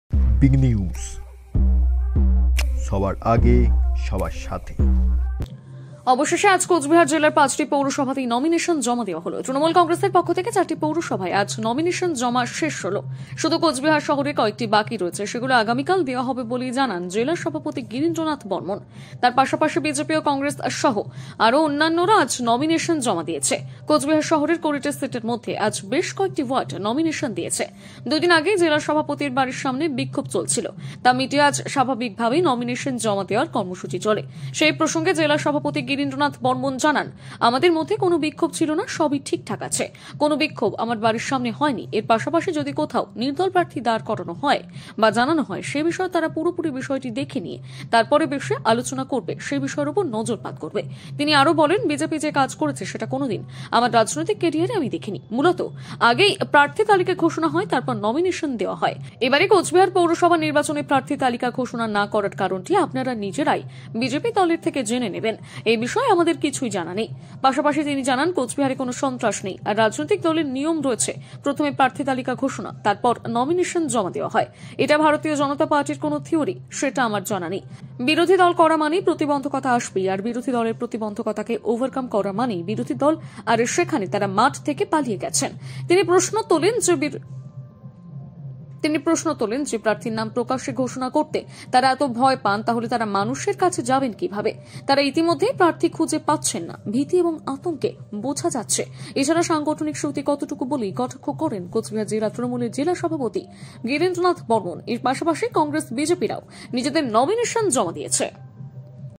কোচবিহার ll তৃণমূল কংগ্রেসের পক্ষ থেকে চারটি পৌরসভায় আজ নমিনেশন জমা শেষ করা হলো।
অবশেষে আজ কোচবিহার জেলার পাঁচটি পৌরসভা তেই নমিনেশন জমা দেওয়া হল। তৃণমূল কংগ্রেসের পক্ষ থেকে চারটি পৌরসভায় আজ নমিনেশন জমা শেষ করা হলো। শুধু কোচবিহারে শহরেই কয়েকটি বাকি রয়েছে সেগুলি আগামীকাল দেওয়া হবে বলে জানান জেলা সভাপতি গিরীন্দ্রনাথ বর্মন।

তার পাশাপাশি বিজেপি ও কংগ্রেস সহ আরো অন্যান্যরা ও আজ নমিনেশন জমা দিয়েছে। কোচবিহার শহরের কুড়িটি সিটের মধ্যে আজ বেশ কয়েকটি ওয়ার্ড নমিনেশন জমা দিয়েছে।

দুদিন আগেই জেলা সভাপতির বাড়ির সামনে বিক্ষোভ চলছিল তা মিটিয়ে আজ স্বাভাবিকভাবেই নমিনেশন জমা দেওয়া কর্মসূচি চলে । সেই প্রসঙ্গে জেলা সভাপতি গিরীন্দ্রনাথ বর্মন জানান আমাদের মধ্যে কোনো বিক্ষোভ ছিল না সবই ঠিকঠাক আছে। কোন বিক্ষোভ আমার বাড়ির সামনে হয়নি। এর পাশাপাশি যদি কোথাও নির্দল প্রার্থী দাঁড় করানো হয় বা জানানো হয় সে বিষয়ে তারা পুরোপুরি বিষয়টি দেখে নিয়ে তারপরে বসে আলোচনা করে সে বিষয়টির উপর নজর পাত করবেন।

তিনি আরো বলেন বিজেপি যে কাজ করেছে সেটা কোনদিন আমার রাজনৈতিক ক্যারিয়ারে আমি দেখিনি। মূলত আগেই প্রার্থী তালিকা ঘোষণা হয় তারপরেই নমিনেশন জমা দেওয়া হয়। এবারে কোচবিহার পৌরসভার নির্বাচনে প্রার্থী তালিকা ঘোষণা না করা কারণটি আপনারা নিজেরাই বিজেপি দলের থেকে জেনে নিবেন এ বিষয়ে আমাদের কিছুই জানা নেই।

 পাশাপাশি তিনি জানান কোচবিহারে কোনো সন্ত্রাস নেই আর রাজনৈতিক দলের নিয়ম রয়েছে প্রথমে প্রার্থী তালিকা ঘোষণা তারপরেই নমিনেশন জমা দেওয়া হয়। এটা ভারতীয় জনতা পার্টির কোন থিওরি সেটা আমার জানা নেই। বিরোধী দল করা মানেই প্রতিবন্ধক আসবেই আর আর বিরোধী দলের প্রতিবন্ধক ওভারকাম করায় মানে বিরোধীদল আর সেখানে তারা মাঠ থেকে পালিয়ে গেছেন।

 তিনি প্রশ্ন তোলেন যে প্রার্থীর নাম প্রকাশ্যে ঘোষনা করতে তারা এত ভয় পান তাহলে তারা মানুষের কাছে যাবেন কিভাবে। তারা ইতিমধ্যেই প্রার্থী খুঁজে পাচ্ছেন না ভীতি ও আতঙ্ক তাতে বুঝা যাচ্ছে এছাড়া সাংগঠনিক শক্তি কতটুকু বলে কটাক্ষ করেন কোচবিহার জেলা তৃণমূলের জেলা সভাপতি গিরীন্দ্রনাথ বর্মন।

এর পাশাপাশি কংগ্রেস-বিজেপিরাও  নিজেদের নমিনেশন জমা দিয়েছে।